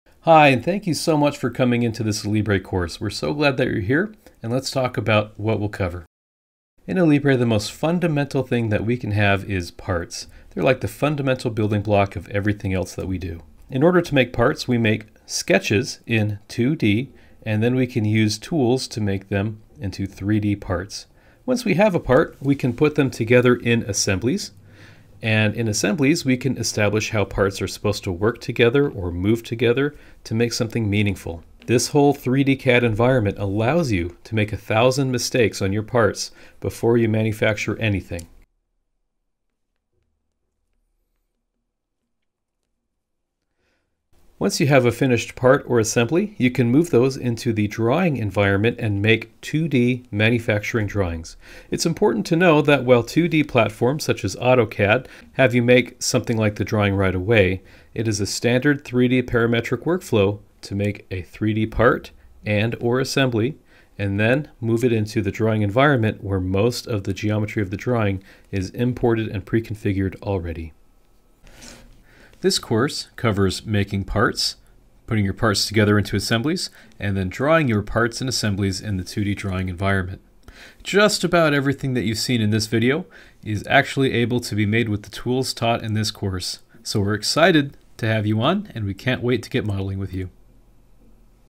Introduction to Alibre | Alibre Beginner Course #1
Lets go through what Alibre is and what modeling looks like!